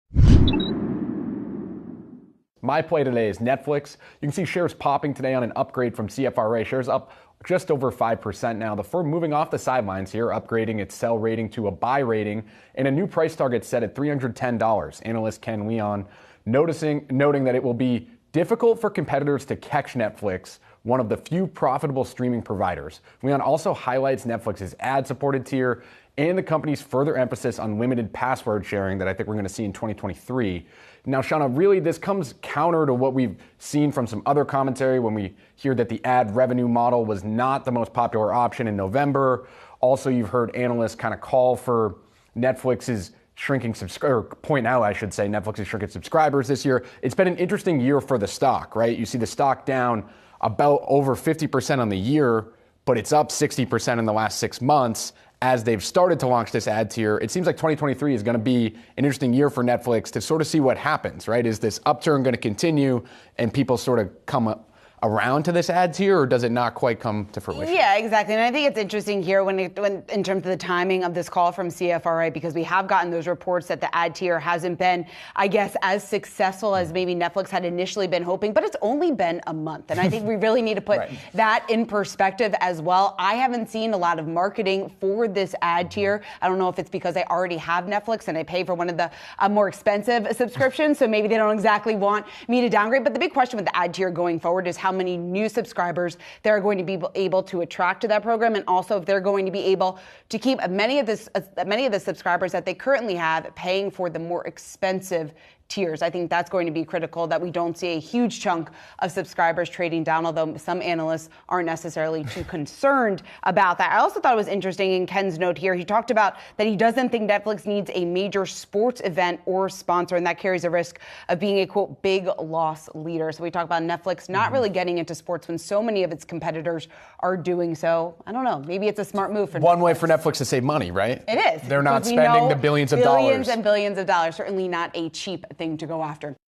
Netflix stock surges following upgrade from CFRA
Netflix stock surges following upgrade from CFRA. Yahoo Finance anchors Jared Blikre, Josh Schafer and Seana Smith discuss shares of Netflix popping after CFRA upgraded the stock to Buy and set a new price target.